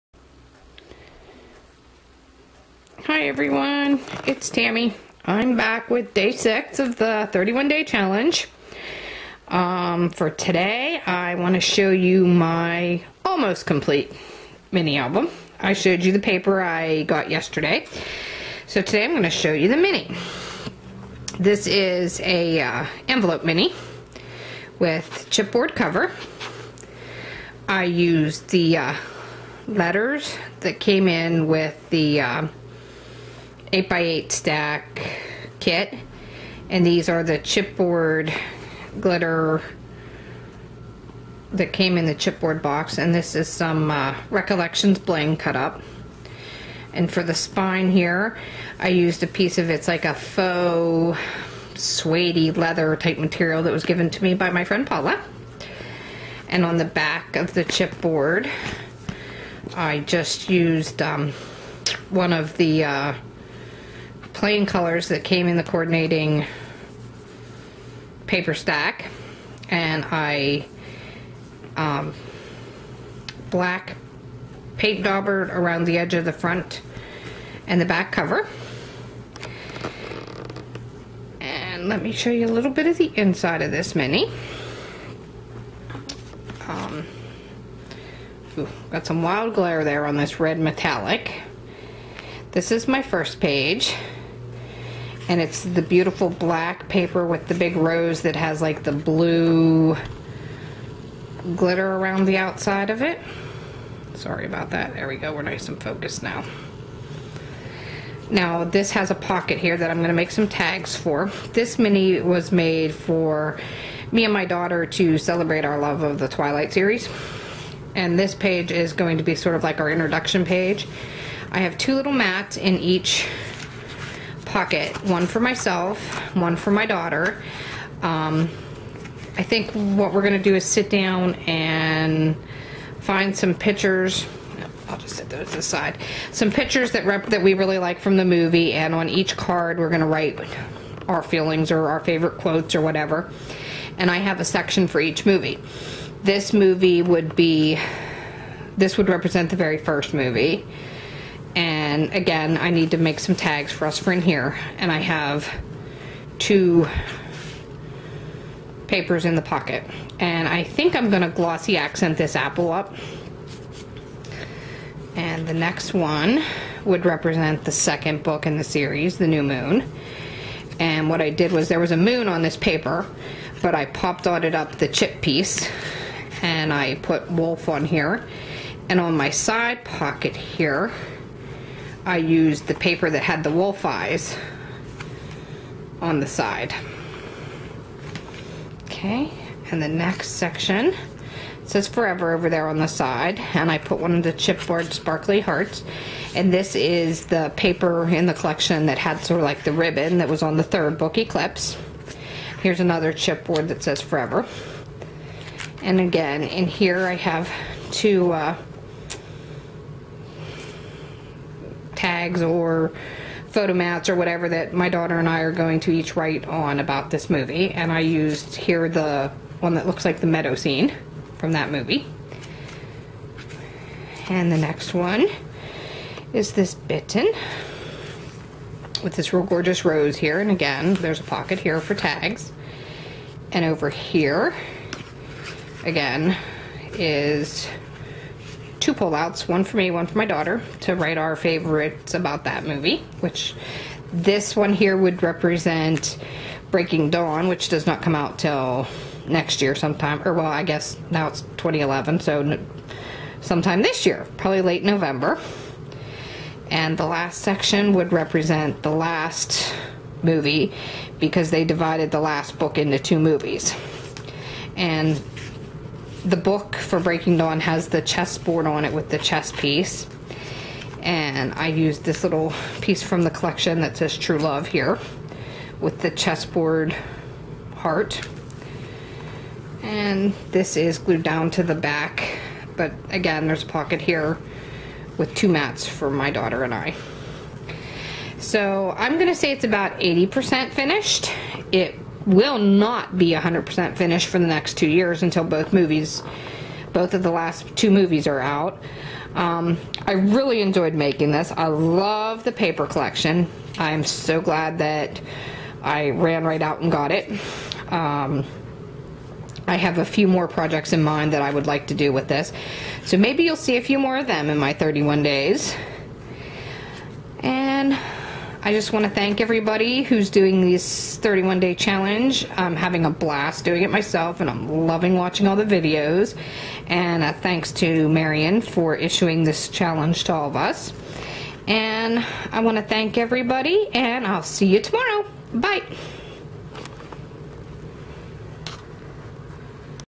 Marion Smith's 31 Day Challenge- Day 6
For my goal for day 6  I got my Twilight mini about 80% done.  I still need to add a few final touches and make tags.  Then my daughter and I need to sit down and find our favorite pics from the movies or books.  I also want to add our favorite quotes or how we felt about each one.  I also saved our movie stubs and will include those in the finished mini.  Thanks again for watching see ya all tomorrow